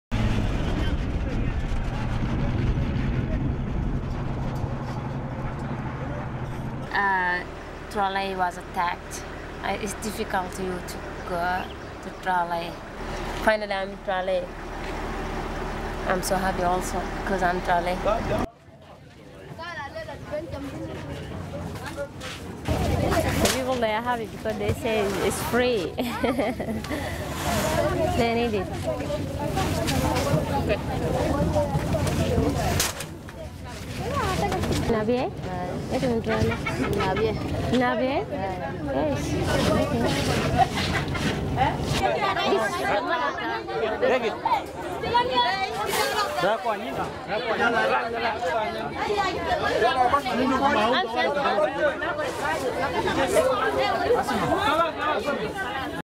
Girifna delivers food to Abyei refugees
The Youth Forum for Peace from Khartoum, organized by Girifna and with help from Sudan Sunrise, delivered emergency relief food to Abyei refugees and other needy to Manute Bol's home town of Turalei in June-July 2011.  Widad Derwish from Khartoum, and others featured in their video, refused to turn back despite an attack on Turalei.